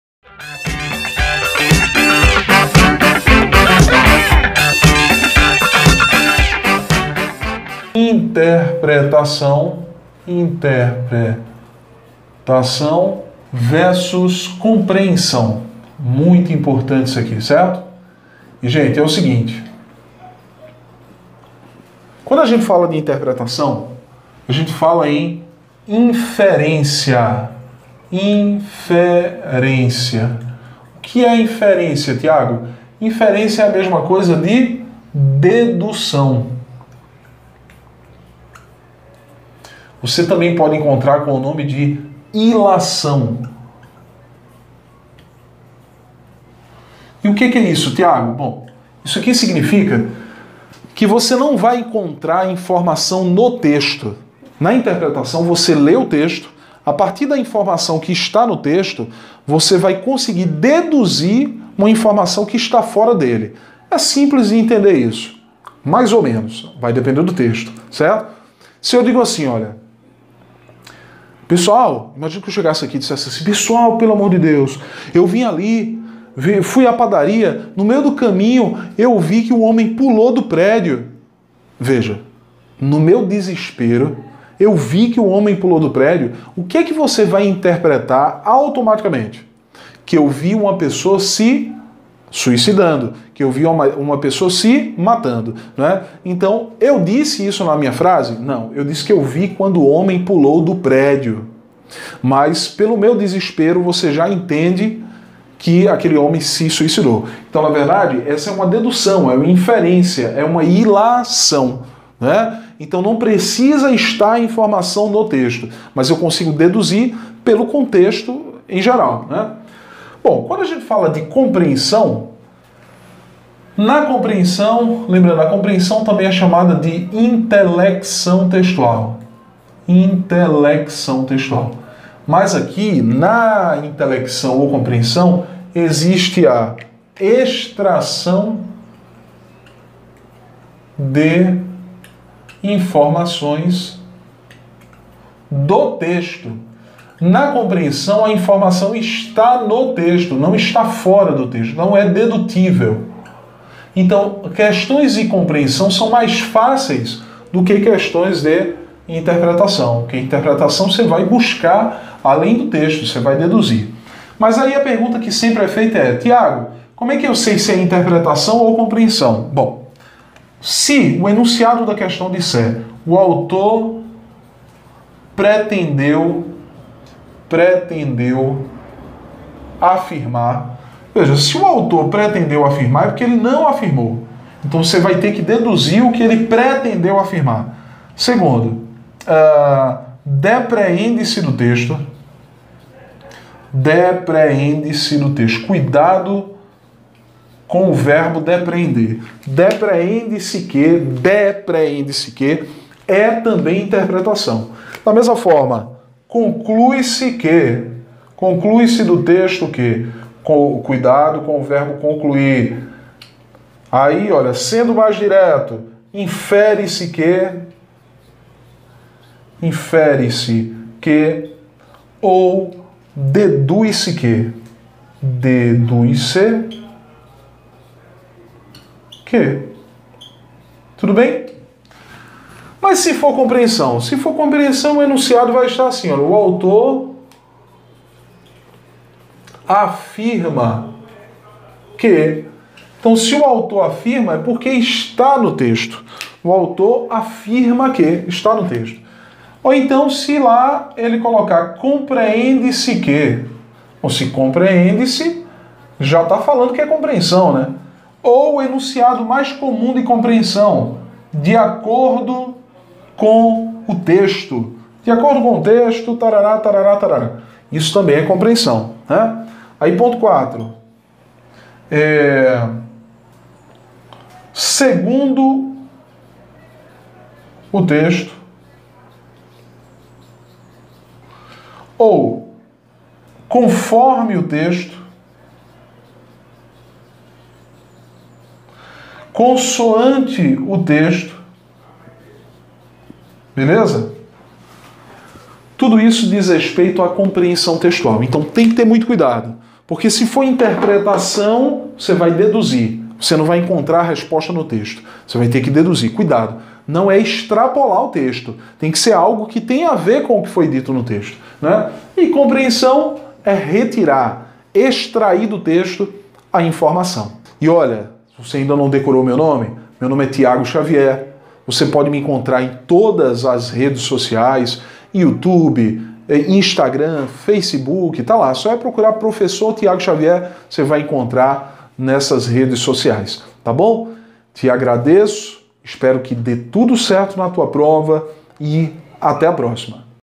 INTERPRETAÇÃO x COMPREENSÃO
Aula diferenciando INTERPRETAÇÃO de COMPREENSÃO